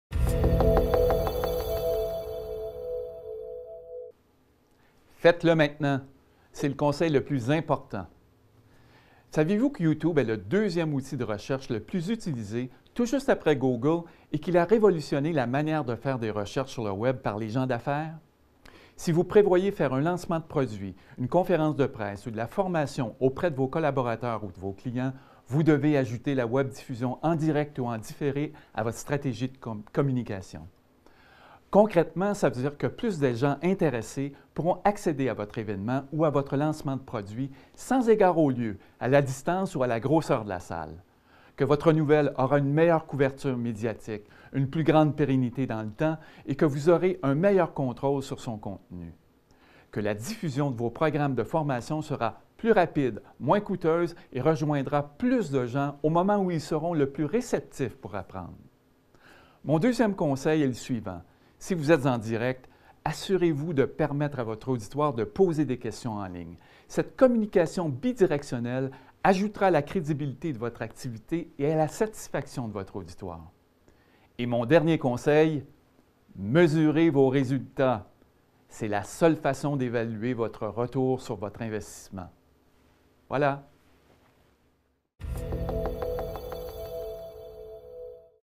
3 conseils pour réussir une webdiffusion
Claude Boudreau, président de Cybermediacom Inc. présente trois conseils pour réussir votre webdiffusion.